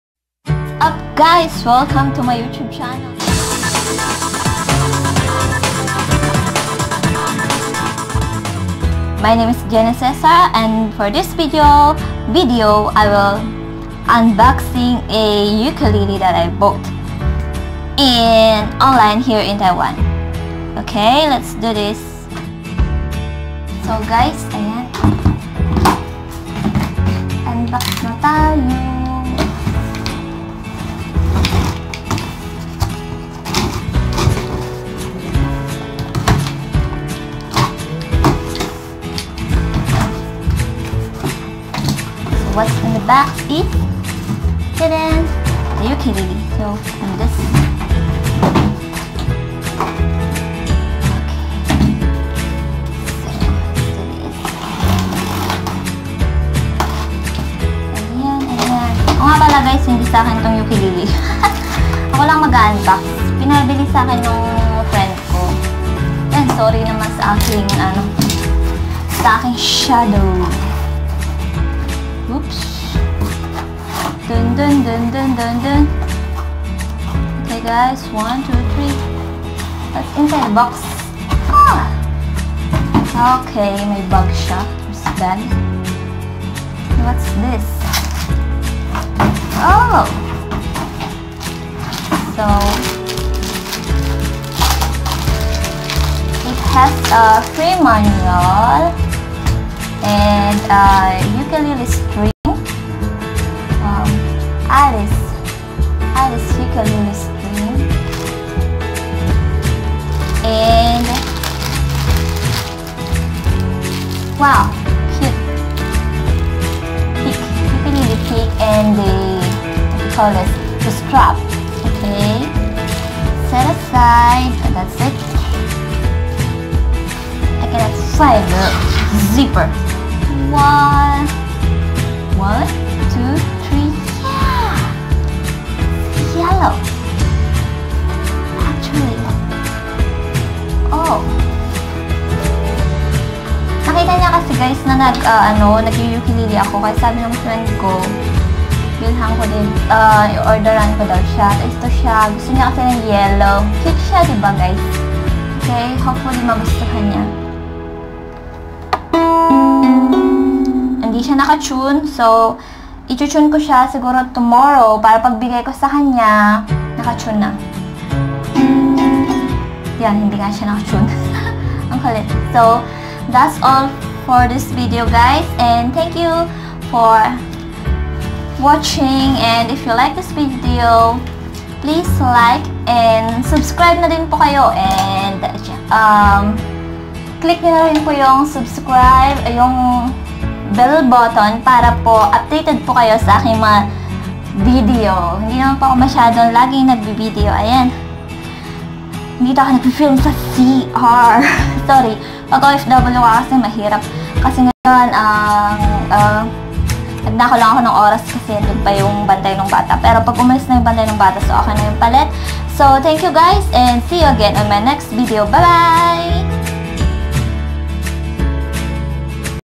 KAKA 21` UKULELE UNBOXING😍
Ukulele unboxing tayo guys!😍 I ordered this ukulele for my friend. And I ask permission to use it for this video😃.
Sorry guys sa cr ako nkapg film , wala kc mataguan😃.